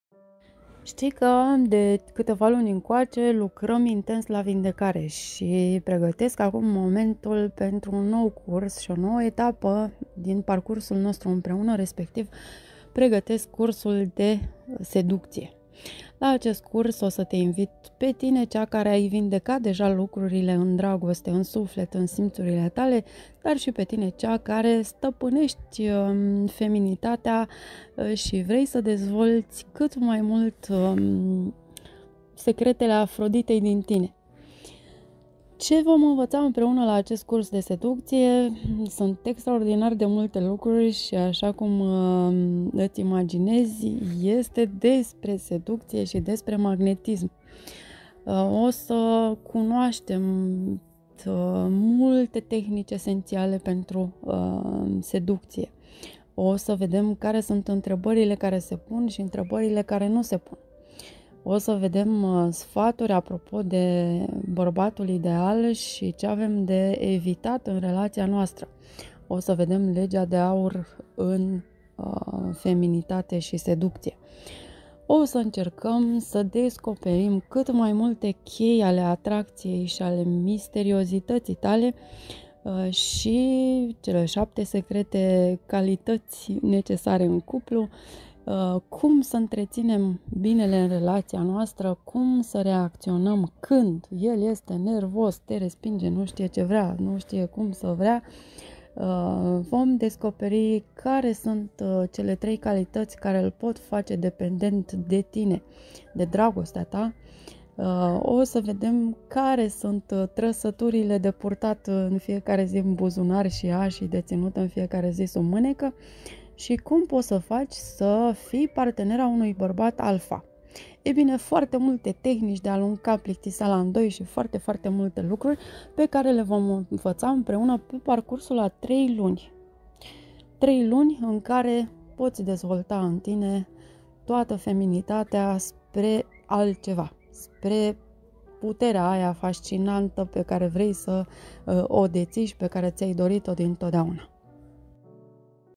Seducție și Mister, Curs Online: Învata sa fii seducatoare si misterioasa
Un mod de a te face fericită chiar și pe tine! Atunci când ești seducătoare, sigură pe tine, zâmbitoare și plină de resurse, atunci când atragi tot ce îți dorești și nu ceea ce atrăgeai demult și negativ, atunci ești plină de viață și de fericire chiar tu, dar și cei care te înconjoară!
Îți las un link unde poți găsi mai multe materiale despre curs, despre magnetism și despre seducție: